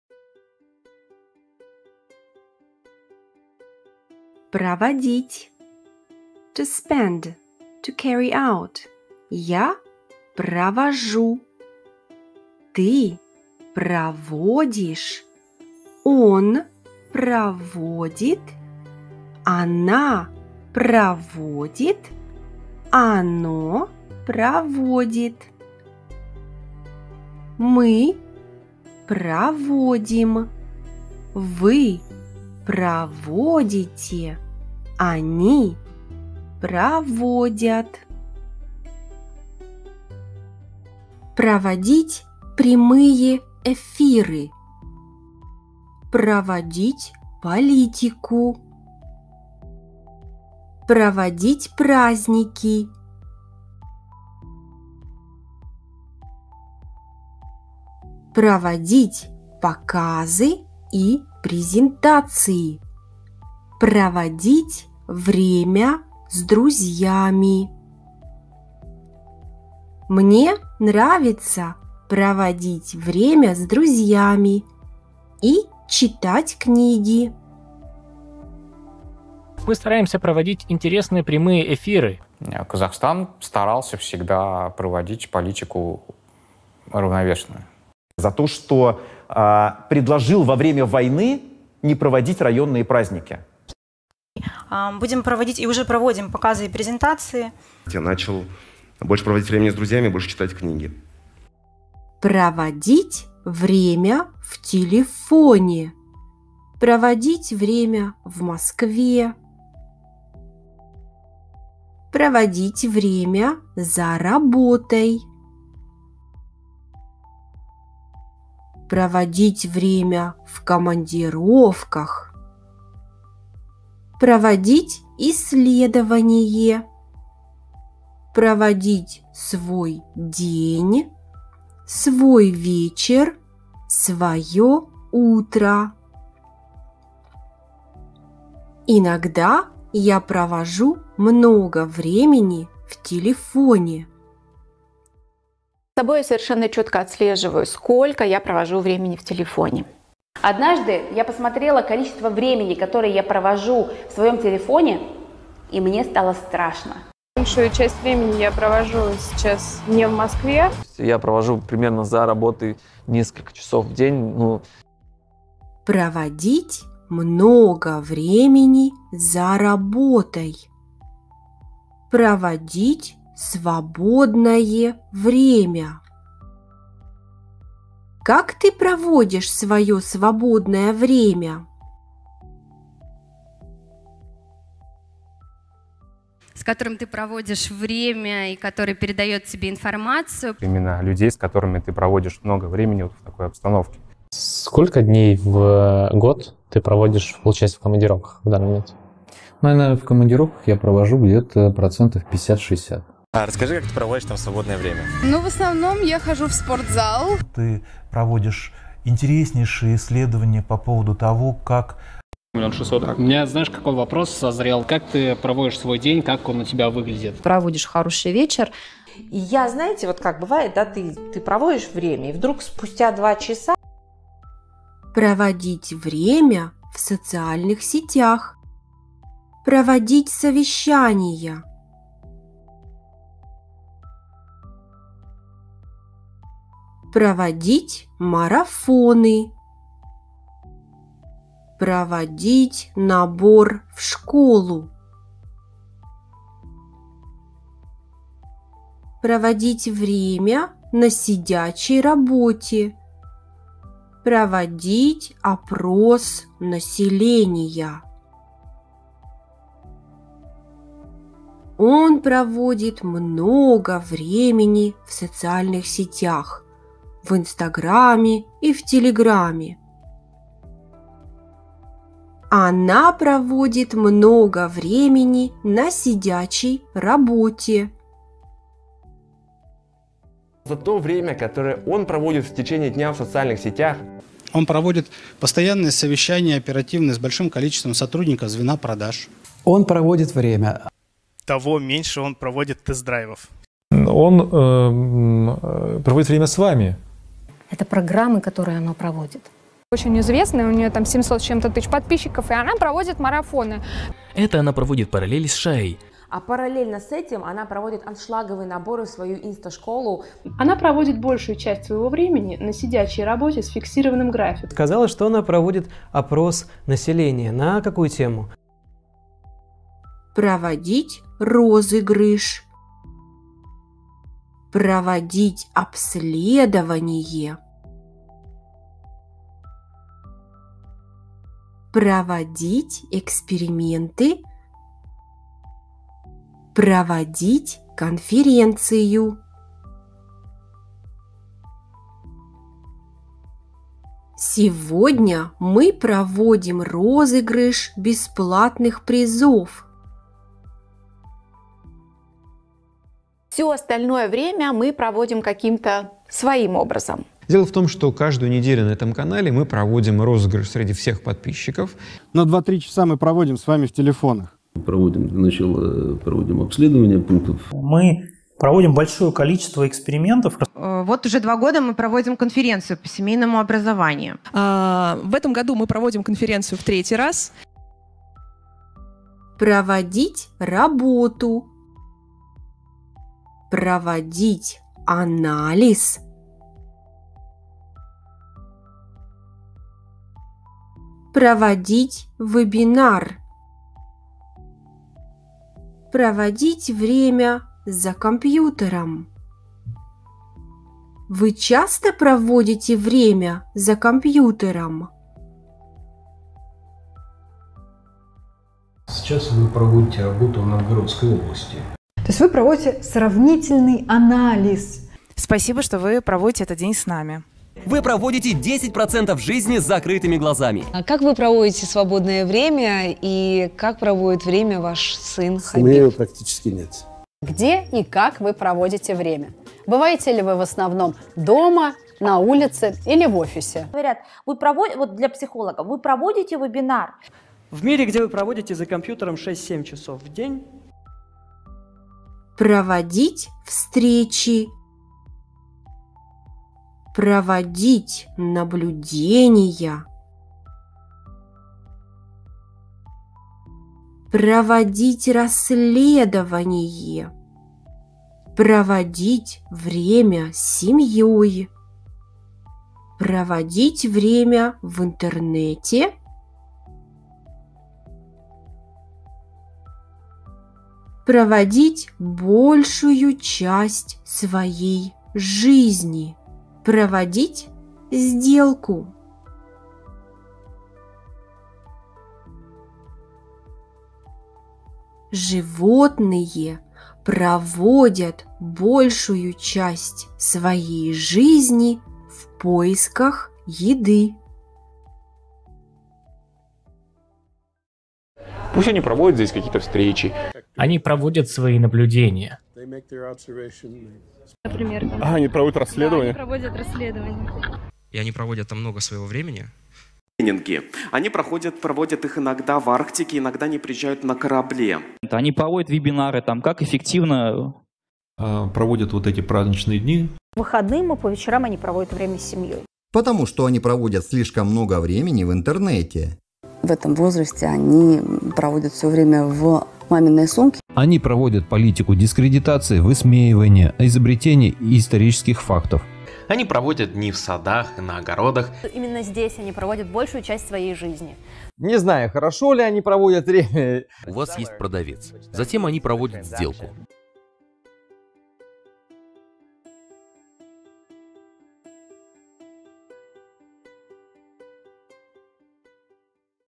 Проводить. To spend. Conjugate Russian verbs in context with a lot of examples.
Learn how to say " to do a reserch", "to investigate", "to hold prize drawings", to conduct a population survey" etc. by using verb " Проводить".

Would you like to learn Russian online with a professional native speaker in a small group or individually ?